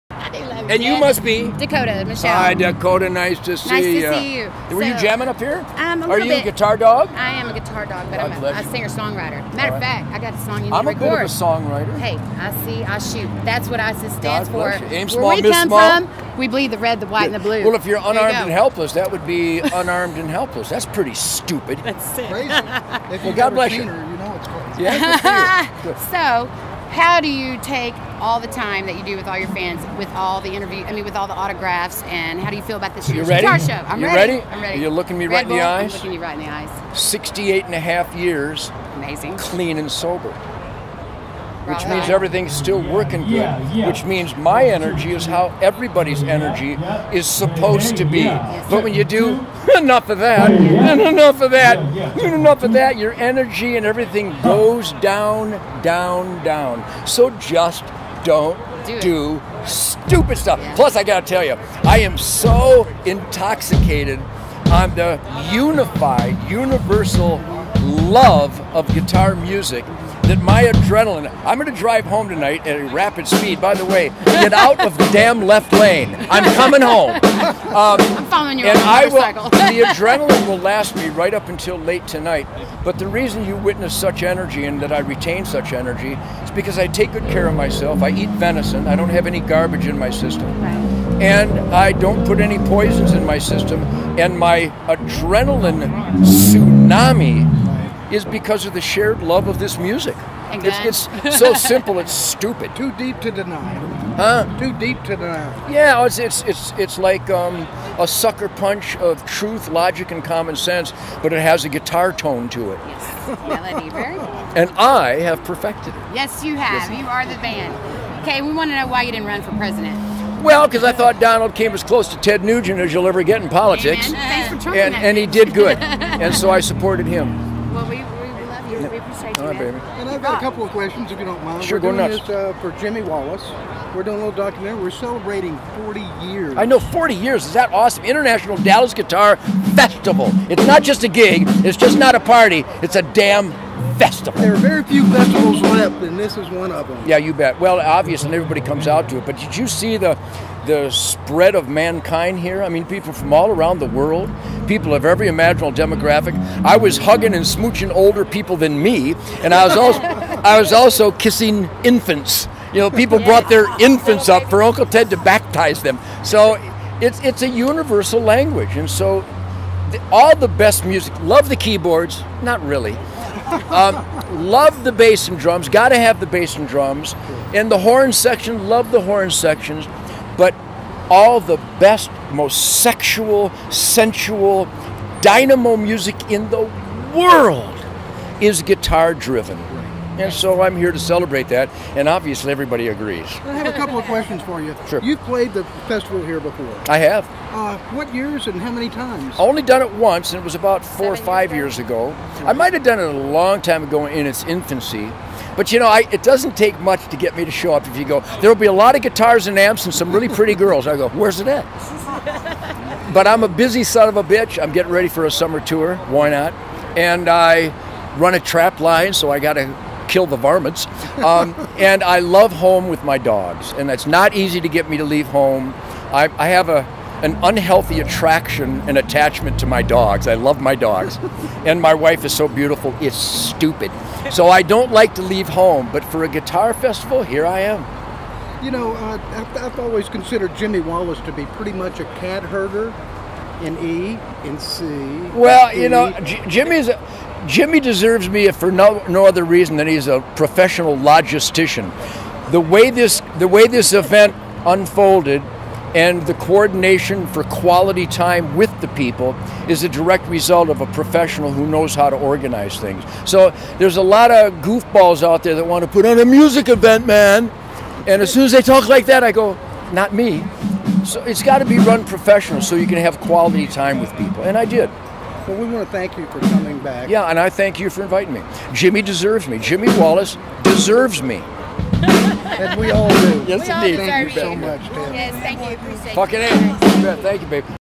Ted Nugent Interviewed at the 40th annual 2017 Dallas International Guitar Show
Ted Nugent took a moment to talk with us before going up on stage and then tearing it up at the 40th annual 2017 Dallas International Guitar Show!  With Ms. Dakota and J.W. Hammett.